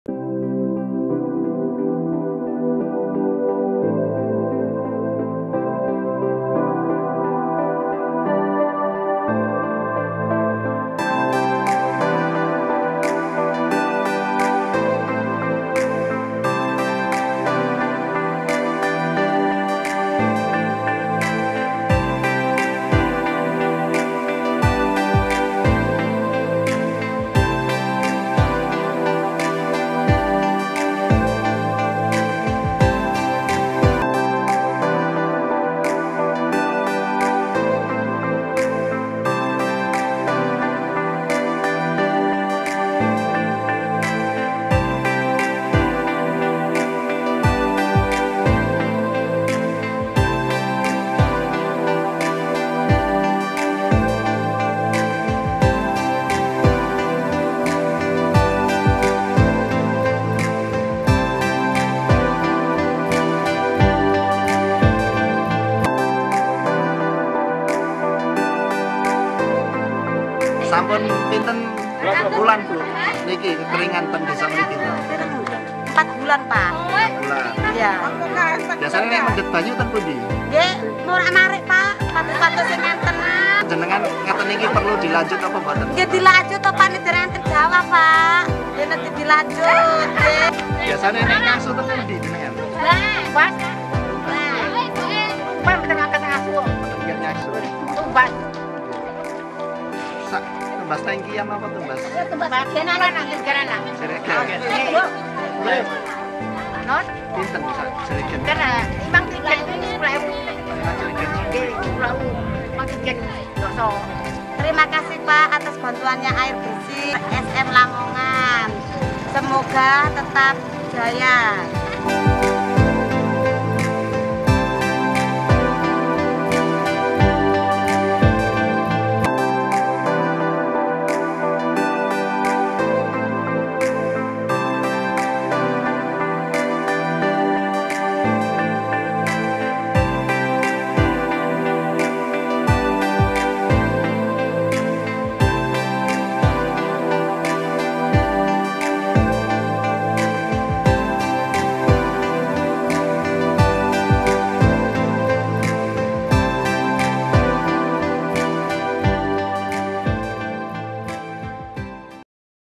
Bakti Sosial Bantuan Air Bersih RS Muhammadiyah Lamongan
Baksos Air Bersih bagi masyarakat yang wilayahnya mengalami kekeringan akibat kemarau panjang di tahun 2019, oleh RS Muhammadiyah Lamongan